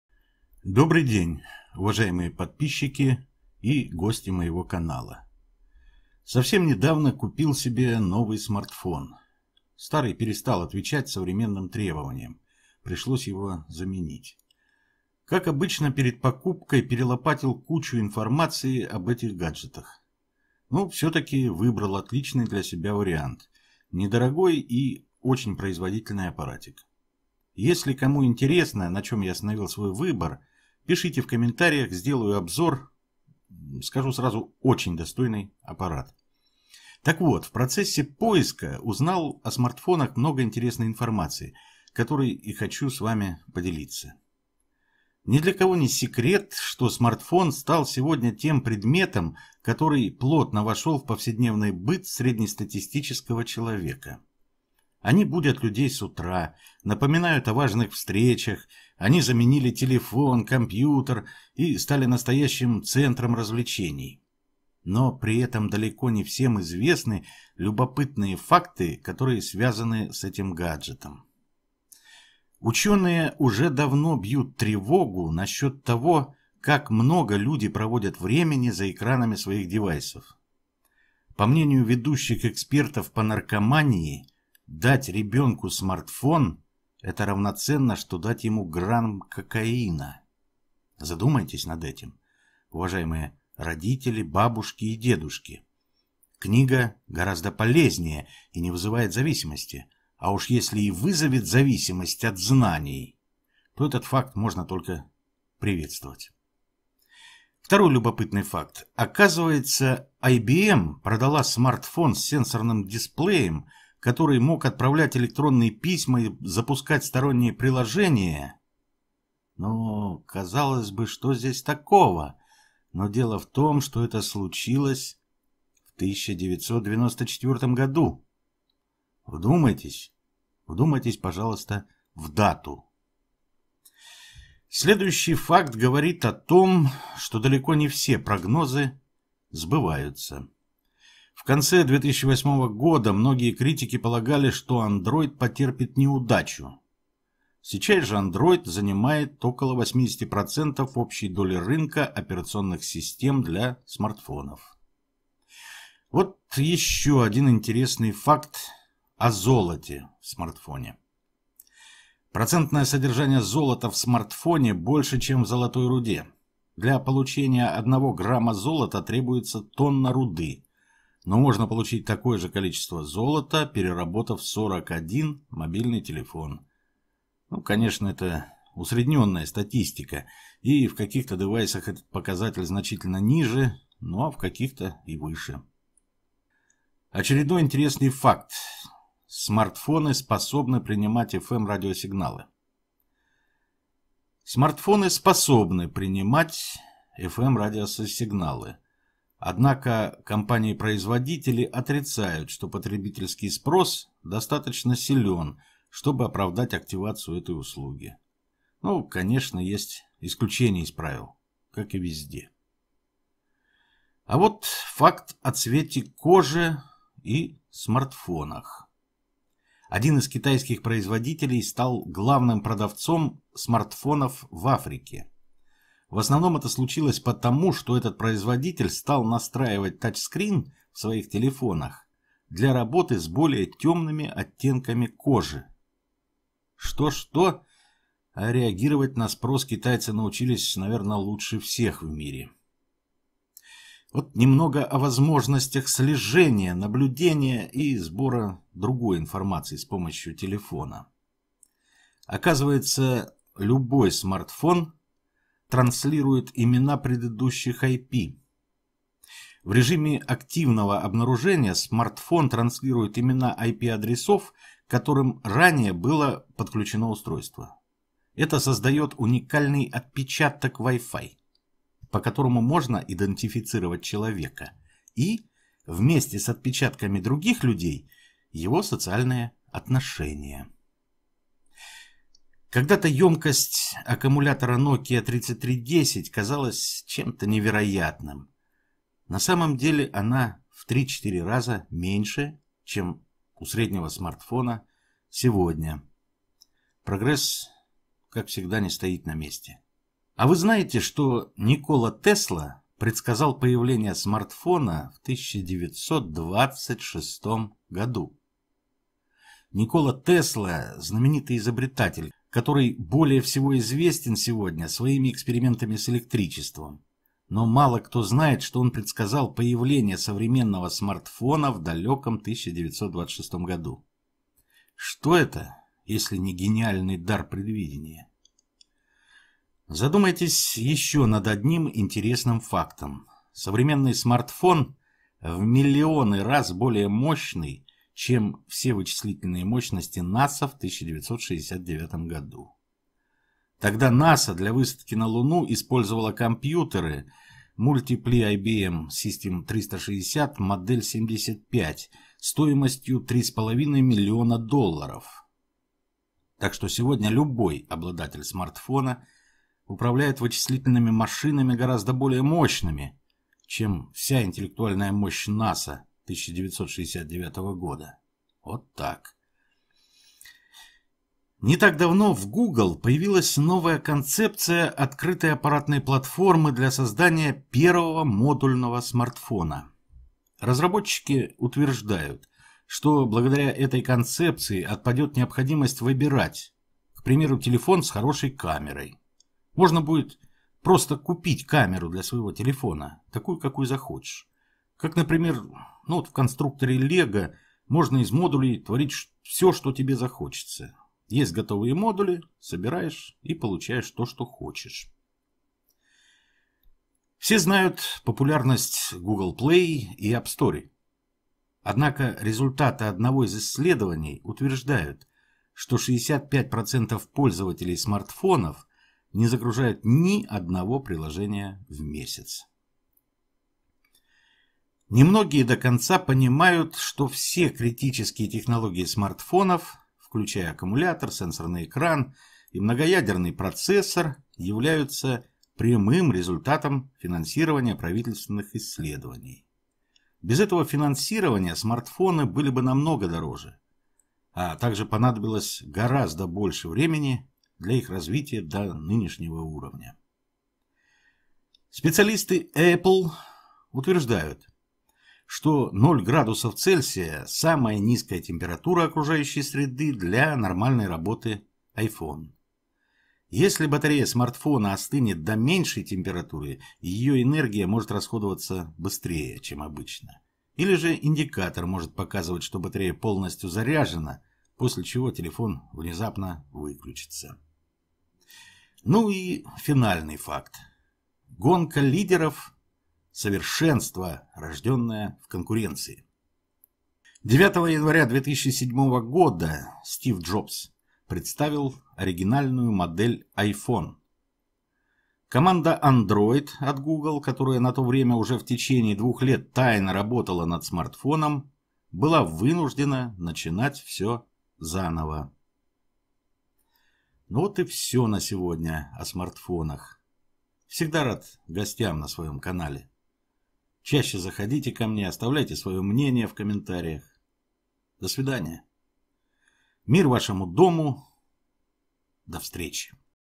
Смартфоны История и действительность
Ученые уже давно бьют тревогу насчет того, как много люди проводят времени за экранами своих девайсов. По мнению ведущих экспертов по наркомании, «дать ребенку смартфон – это равноценно, что дать ему грамм кокаина». Задумайтесь над этим, уважаемые родители, бабушки и дедушки, книга гораздо полезнее и не вызывает зависимости, а уж если и вызовет зависимость от знаний, то этот факт может только радовать.
Второй любопытный факт, оказывается IBM продала смартфон с сенсорным дисплеем, который мог отправлять электронные письма и запускать сторонние приложения. Казалось бы, что здесь такого. Но дело в том, что это случилось в 1994 году. Вдумайтесь пожалуйста в дату!
Следующий факт говорит о том, что далеко не все прогнозы сбываются. В конце 2008 года многие критики полагали, что Android потерпит неудачу. Сейчас же Android занимает около 85% общей доли рынка операционных систем для смартфонов.
Вот еще интересный факт о золоте в смартфоне.
Процентное содержание золота в смартфоне больше, чем в золотой руде. Для получения 1 грамма золота требуется тонна руды. Но можно получить такое же количество золота, переработав 41 мобильный телефон. Конечно это усредненная статистика и в каких-то девайсах этот показатель значительно ниже, а в каких-то и выше.
Очередной интересный факт: смартфоны способны принимать FM-радиосигналы.
Смартфоны способны принимать FM-радиосигналы. Однако компании-производители отрицают, что потребительский спрос достаточно силен, чтобы оправдать активацию этой услуги, но конечно есть исключения из правил, как и везде.
А вот  факт о цвете кожи и смартфонах.
Один из китайских производителей стал главным продавцом смартфонов в Африке. В основном это случилось потому, что этот производитель стал настраивать татч скрин в своих телефонах для работы с более темными оттенками кожи. Что-что а реагировать на спрос китайцы научились лучше всех.
Немного о возможностях слежения, наблюдения и сбора другой информации с помощью телефона… Оказывается любой смартфон транслирует имена «предыдущих» AP.
В режиме активного обнаружения смартфон транслирует имена AP, к которым ранее было подключено устройство. Это создает уникальный «отпечаток» Wi-Fi, по которому можно идентифицировать человека и (вместе с отпечатками других людей) его социальные отношения.
Когда-то емкость аккумулятора Nokia 3310 казалась чем-то невероятным. На самом деле, она в 3-4 раза меньше, чем у среднего смартфона сегодня. Прогресс не стоит на месте.
А вы знаете, что Никола Тесла предсказал появление смартфона в 1926 году?
Никола Тесла – знаменитый изобретатель, который более всего известен сегодня своими экспериментами с электричеством. Но мало кто знает, что он предсказал появление современного смартфона в далеком 1926 году. Что это если ни гениальный дар предвидения?
Задумайтесь еще над одним интересным фактом, современный смартфон в миллионы раз более мощный, чем все вычислительные мощности NASA в 1969 году. Тогда NASA для высадки на Луну использовало компьютеры Multiple IBM System/360 Model 75 стоимостью 3,5 млн долларов. Так что сегодня любой обладатель смартфона управляет вычислительными машинами гораздо более мощными, чем интеллектуальная мощь НАСА 1969 года!
Не так давно в Google появилась новая концепция открытой аппаратной платформы для создания первого модульного смартфона. Разработчики утверждают, что благодаря этой концепции отпадет необходимость выбирать, к примеру, телефон с хорошей камерой, а можно будет купить камеру для своего телефона, как например в конструкторе ЛЕГО можно из модулей творить все что захочется.
Все знают популярность Google Play и App Store. Однако, результаты одного из исследований утверждают, что 65% пользователей смартфонов не загружают ни одного приложений в месяц.
Не многие до конца понимают, что все критические технологии смартфонов, включая аккумулятор, сенсорный экран и многоядерный процессор, являются прямым результатом финансирования правительственных исследований. Без этого финансирования смартфоны были бы намного дороже, а также понадобилось бы много времени для их развития до нынешнего уровня.
Специалисты Apple утверждают, что ноль градусов Цельсия – самая низкая температура окружающей среды для нормальной работы iPhone. Если батарея смартфона остынет до меньшей температуры, ее энергия может расходоваться быстрее, чем обычно, или же индикатор может показывать, что батарея полностью заряжена, после чего телефон внезапно выключится.
Ну и финальный факт: гонка лидеров – совершенство рожденное в конкуренции.
9 января 2007 года Стив Джобс представил оригинальную модель iPhone. Команда Android от Google, которая на то время уже в течение двух лет тайно работала над смартфоном, была вынуждена начинать все заново.